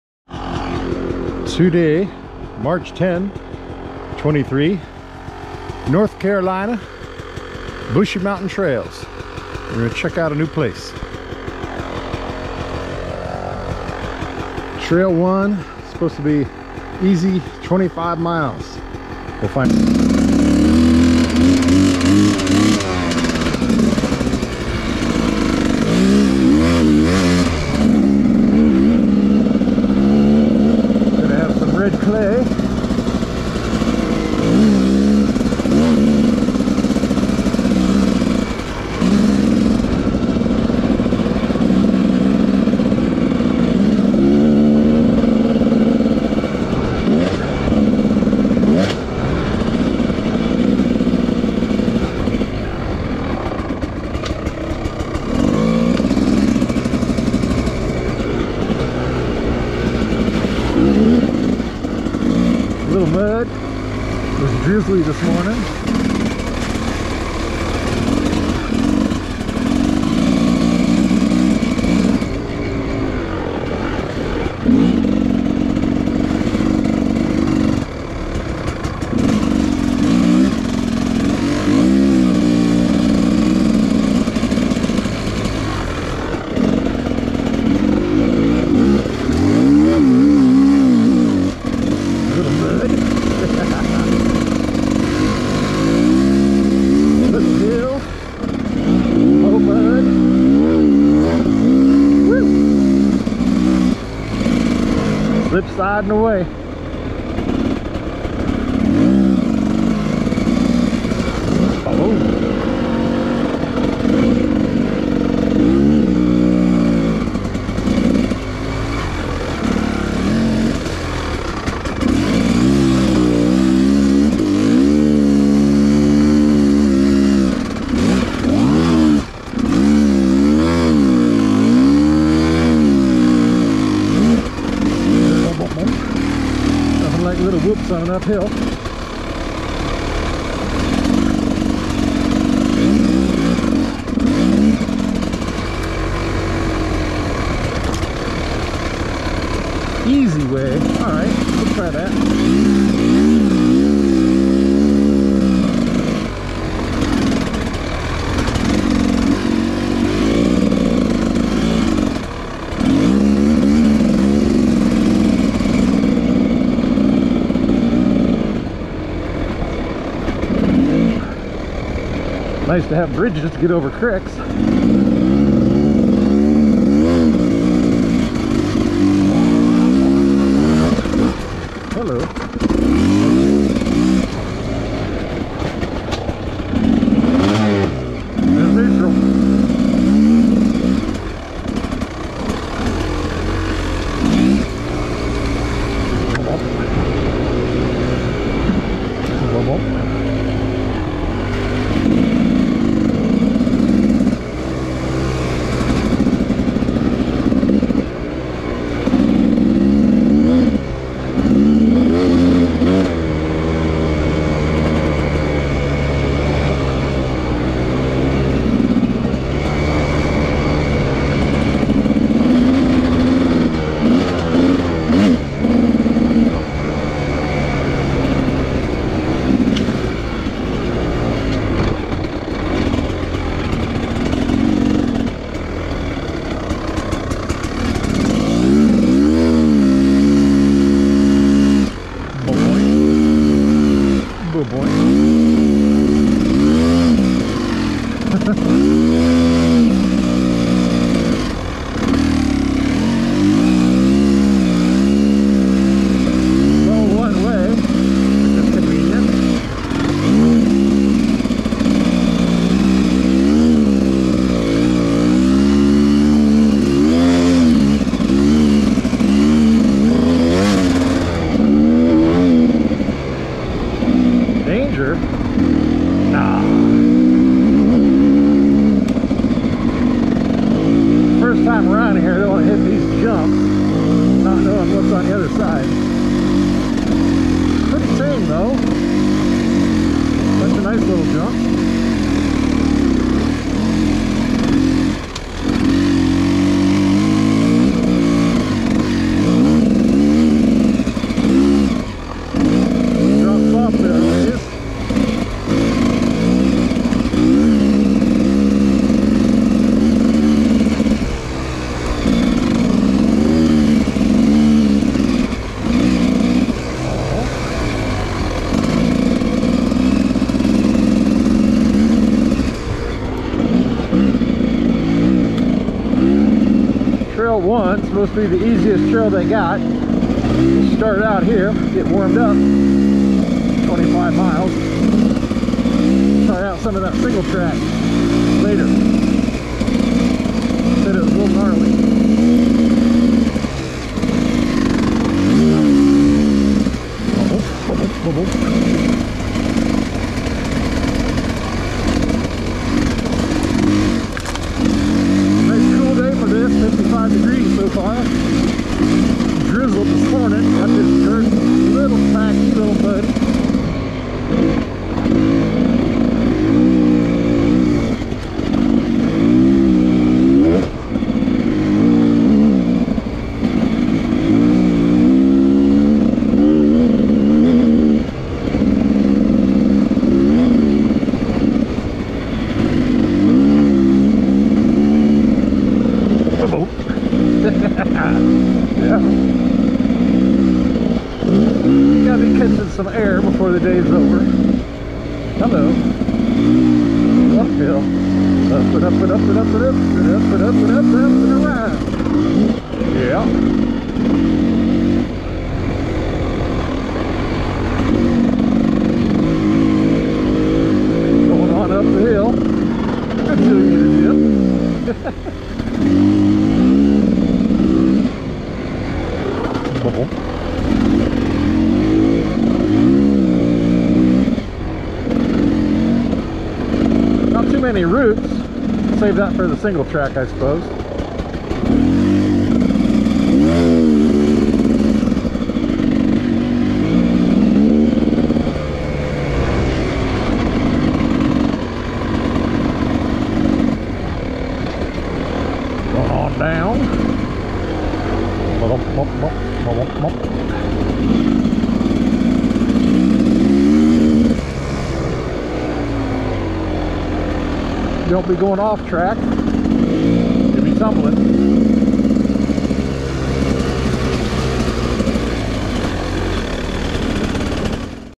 Brushy Mtn pt1
Riding alone Brushy Mtn N Carolina, main trail 3-10-23